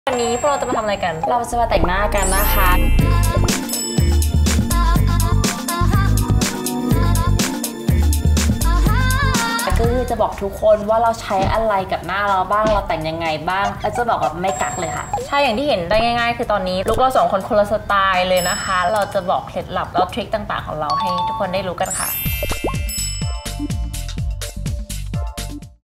[TEASER EP.8] Makeup Tutorial สไตล์แฝดสายแฟ l MillyNikki l Oneplayground
ใครว่าแฝดจะเหมือนกันเสมอไป
Milly & Nikki สาวแฝด 2 สไตล์
จะมาแต่งหน้าในแบบของตัวเอง
เครื่องสำอางชิ้นไหนต้องตำ ต้องโดน

กำเงินให้แน่เตรียม CF
พรุ่งนี้ 6 โมงเย็น ใน
ทาง YouTube